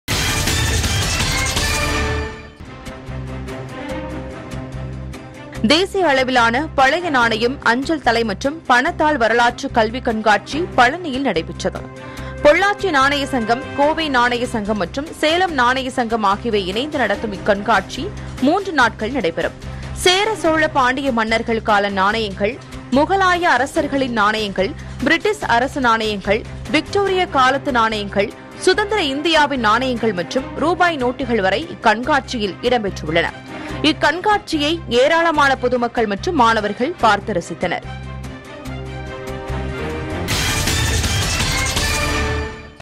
பழனி - தேசிய அளவிலான பழைய நாணயங்களின் கண்காட்சி | செய்தித் துளிகள் | PuthuyugamTV
பழனி - தேசிய அளவிலான பழைய  நாணயங்களின் கண்காட்சி ஏராளமான பொதுமக்கள், மாணவர்கள் பார்த்து ரசிப்பு | செய்தித் துளிகள் | PuthuyugamTV